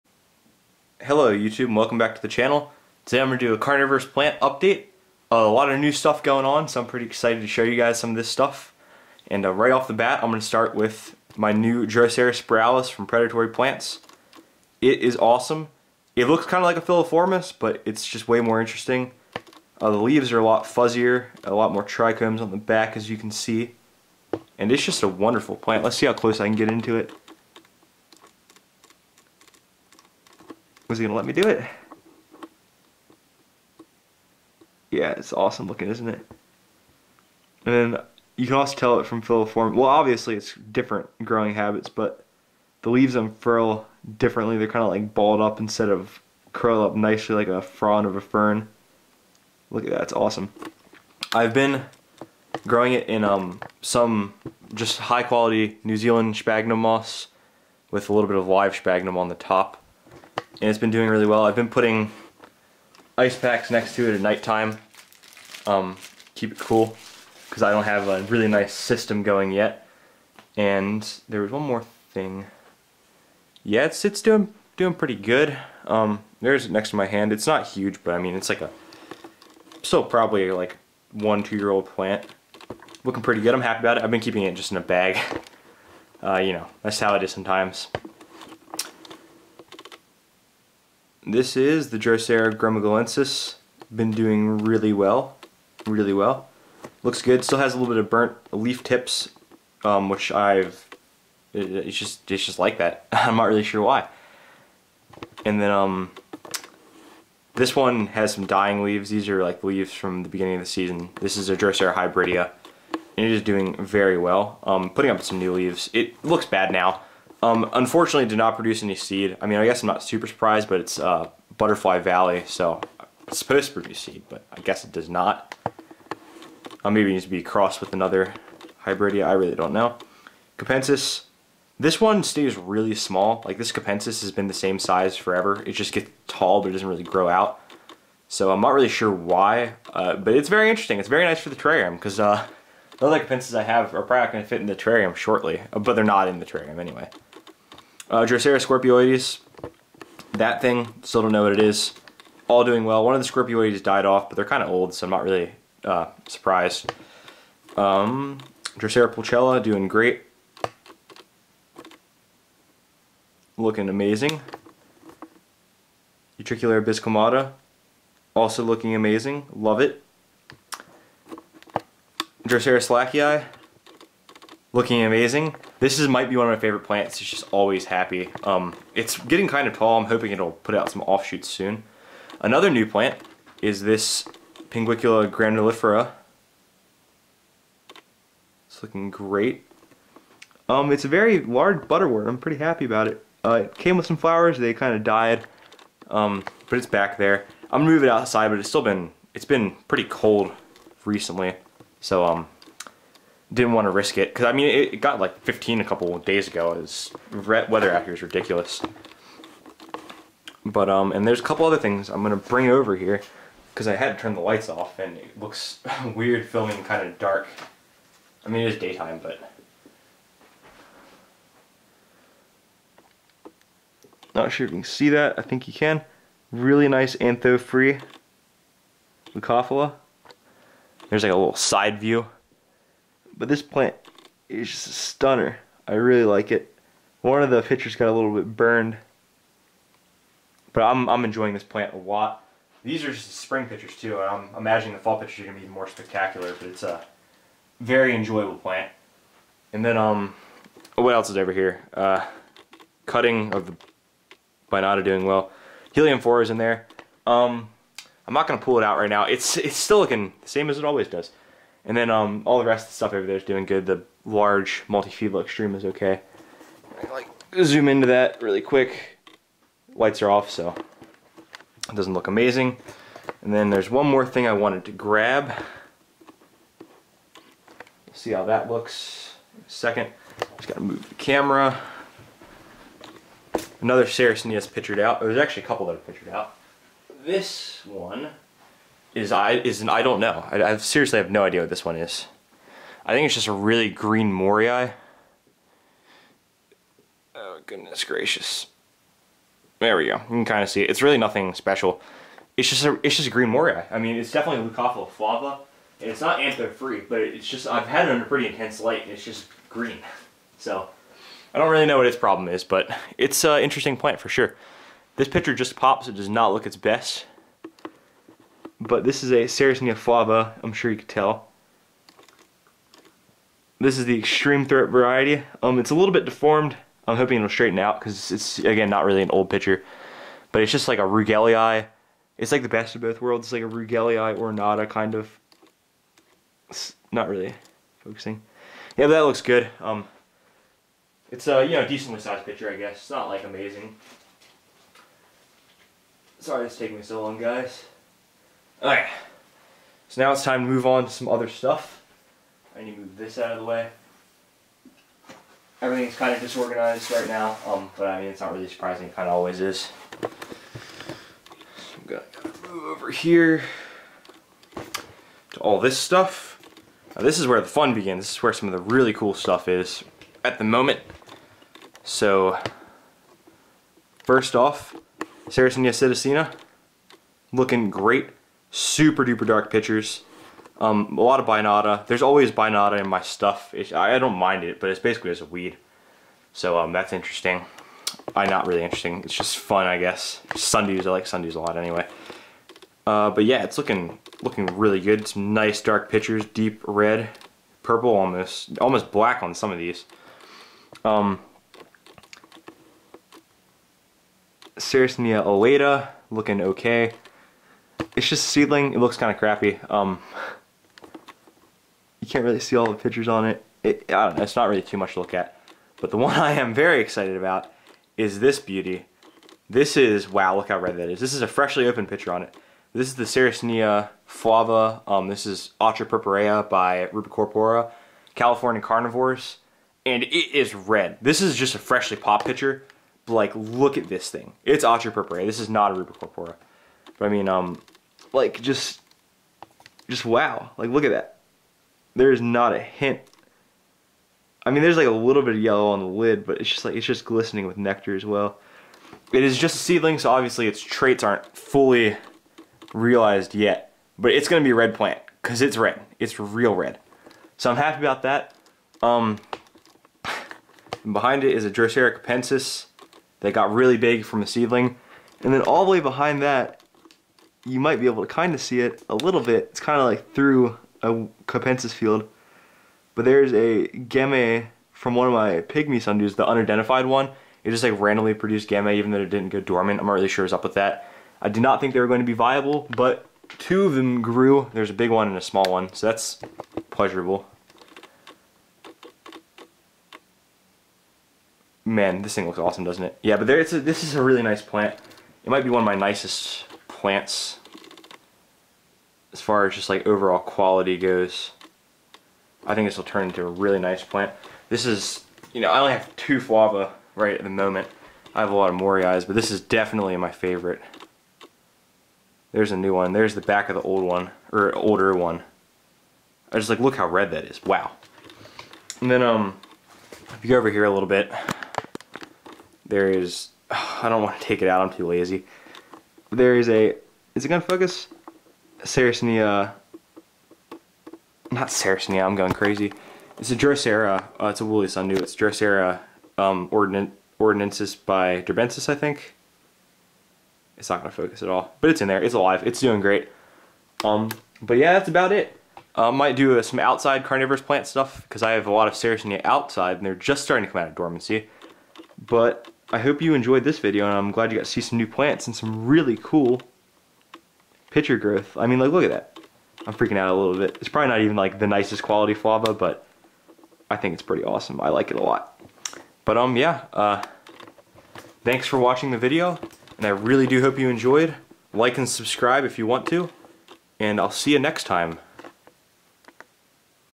Carnivorous Plant Collection Update--New Plants!
An update on all of my indoor carnivorous plants plus some new additions to the collection! Also for some reason I forgot to mention the D.madagascariensis but as you could probably guess it's doing great. I hope you enjoy!